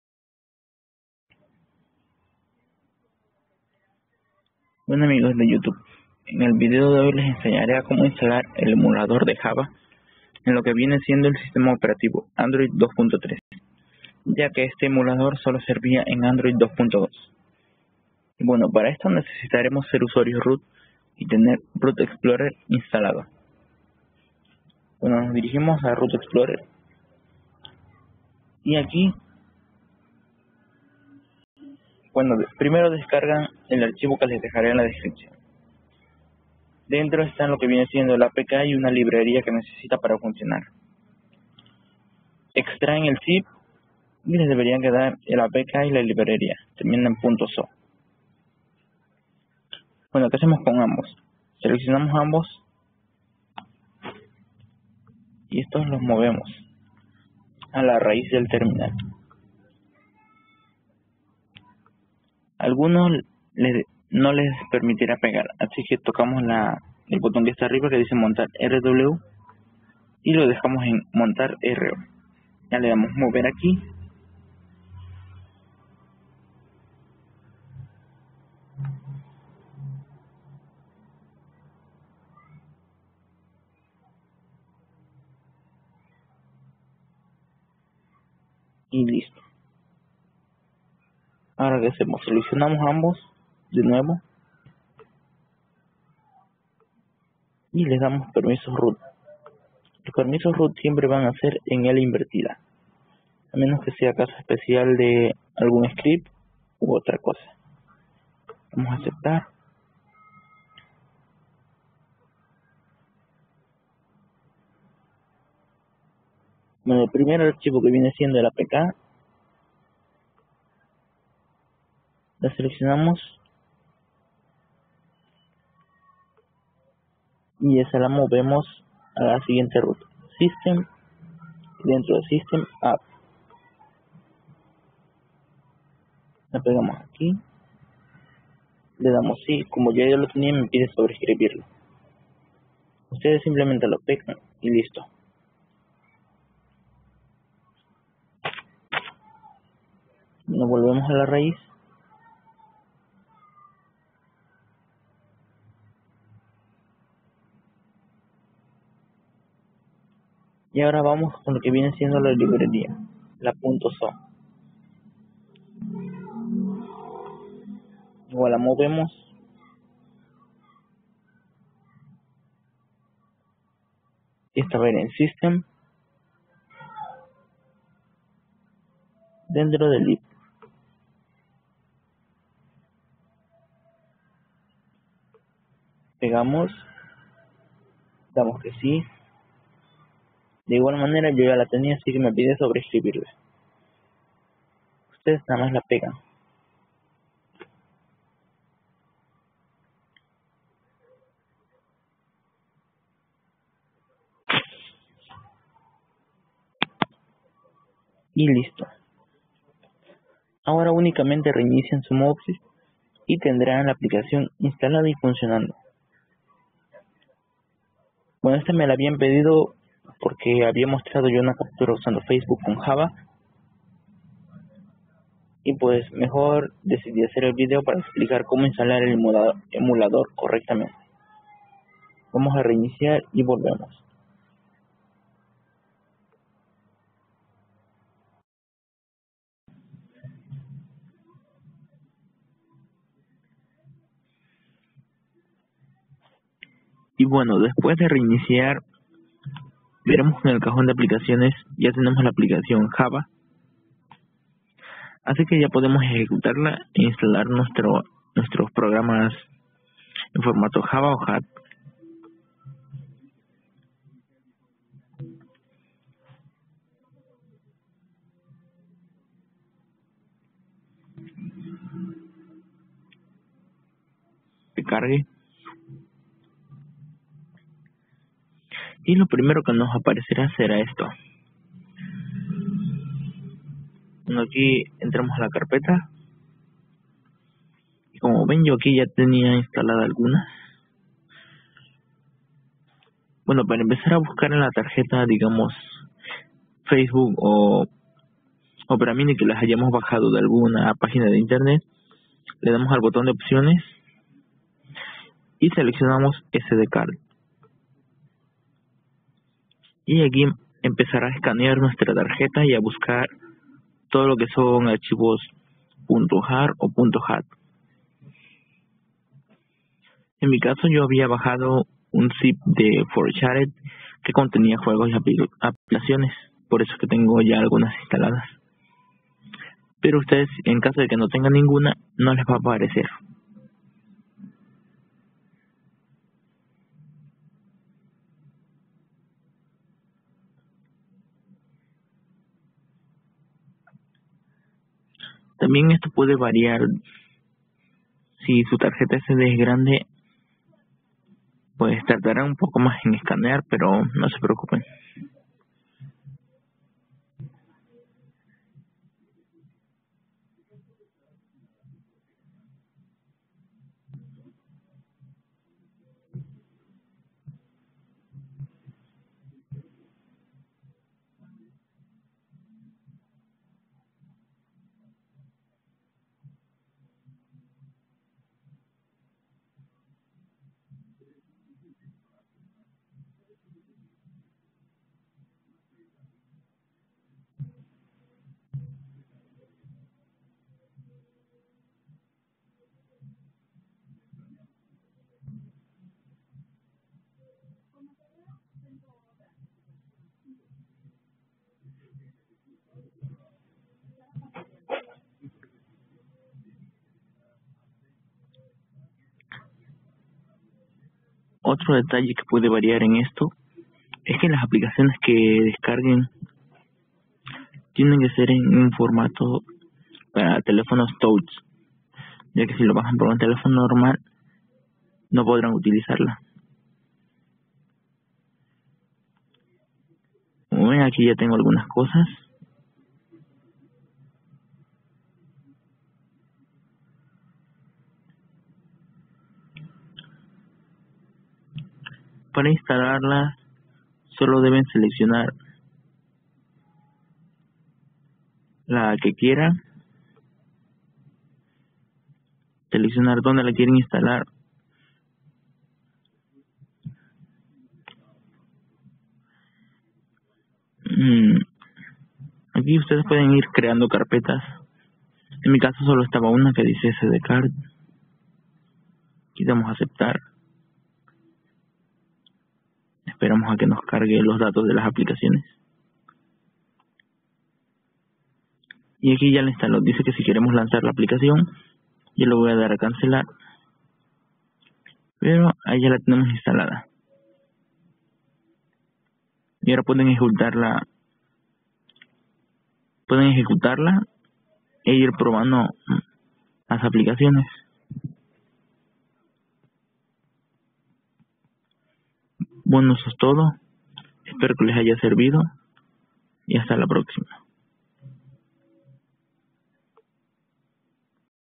Emulador Java para Gingerbread *root*
Aqui les traigo como instalar el emulador de java para android.